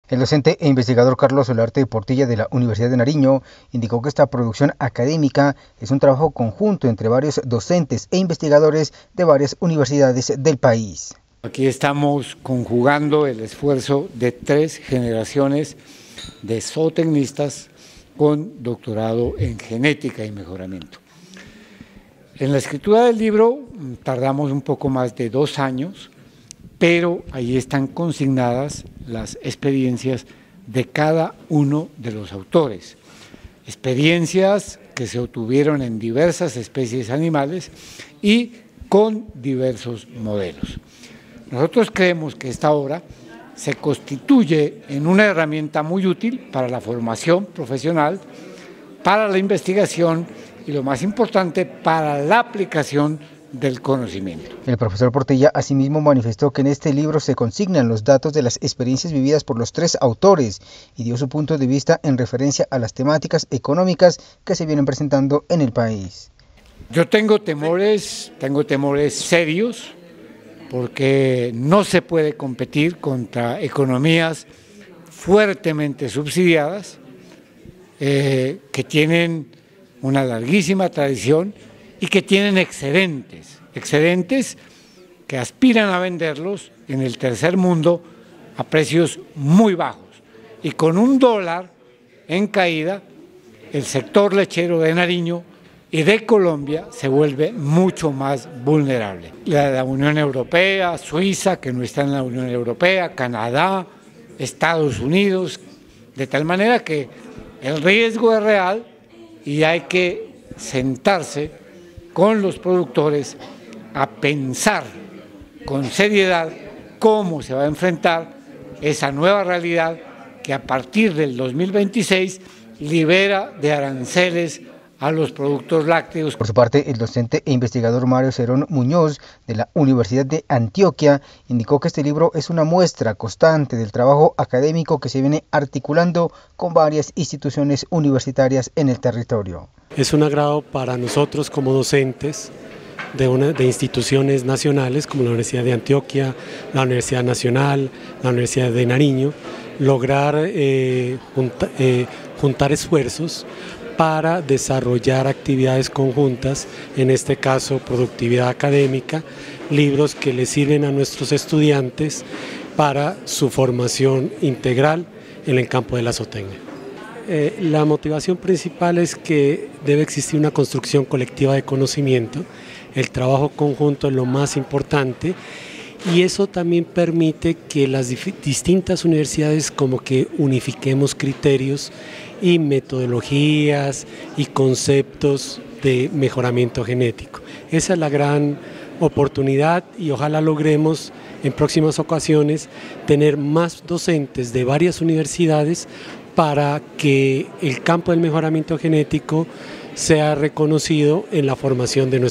CNCNoticiasPasto| lanzamiento del libro "Modelos Lineales para evaluación genética en Animales
CNCNoticiasPasto| Undenar realizó lanzamiento del libro "Modelos Lineales para evaluación genética en Animales"
En el paraninfo de la UDENAR, y con presencia de docentes autores de la Universidad de Nariño, de Antioquia y Universidad Nacional, se realizó el lanzamiento del libro Modelos Lineales para Evaluación Genética en Animales el cual busca ser una fuente de consulta en investigaciones